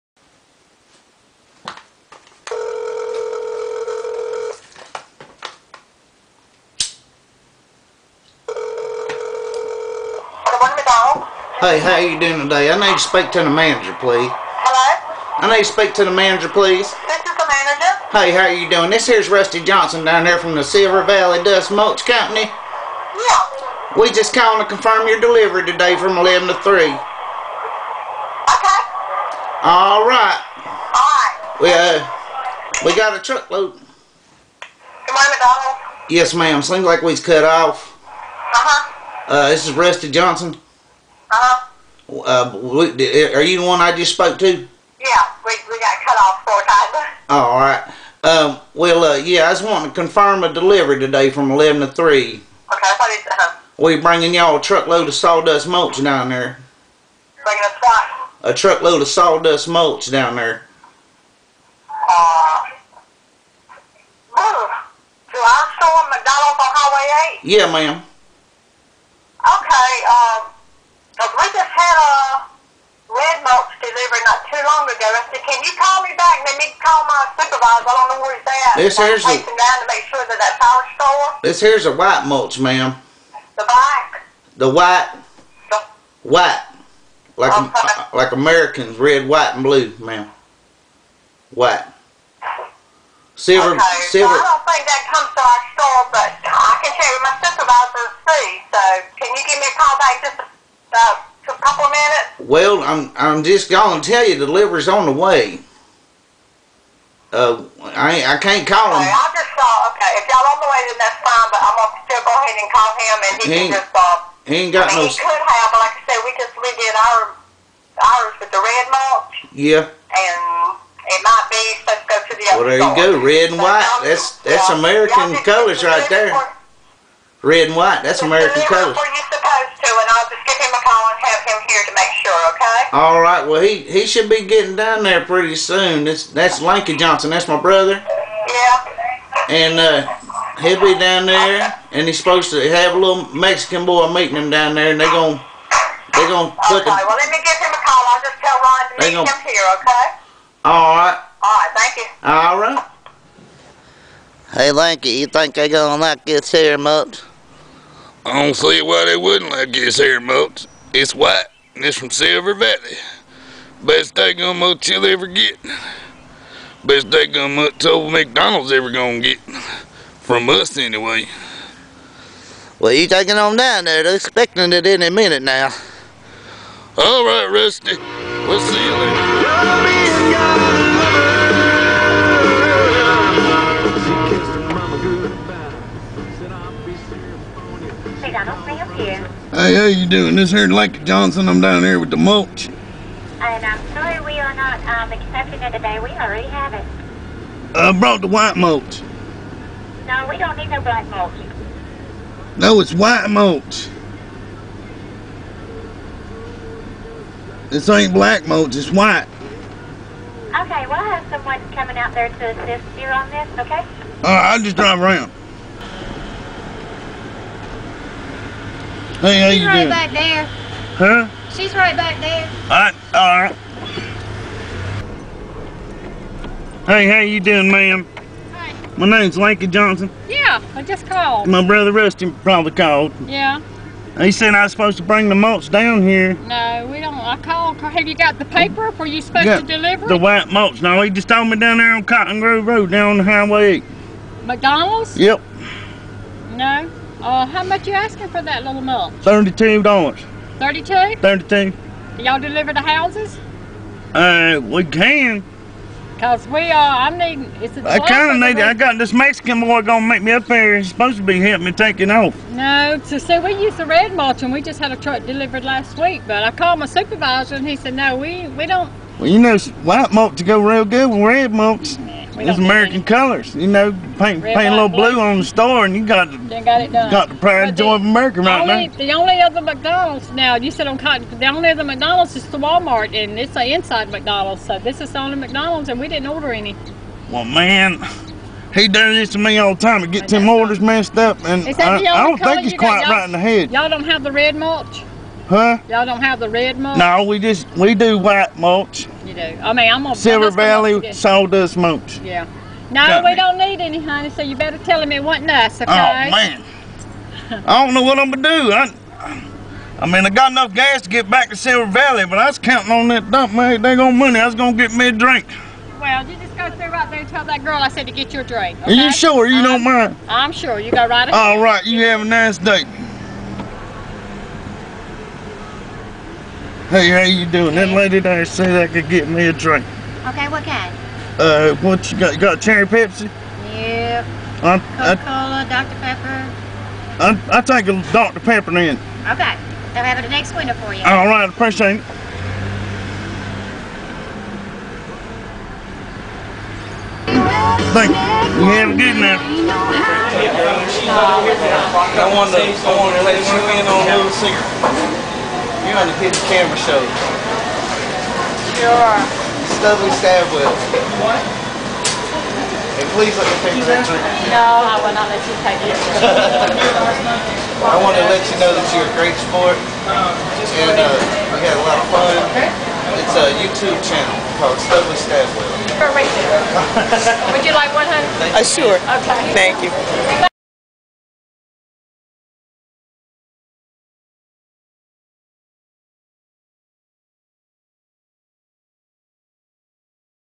Unexpected Mulch Delivery
phone/drive thru prank

Rusty and Lanky Johnson (twin brothers from the Silver Valley Sawdust Mulch Company)deliver mulch to McDonalds... the only problem is they  didn't order any mulch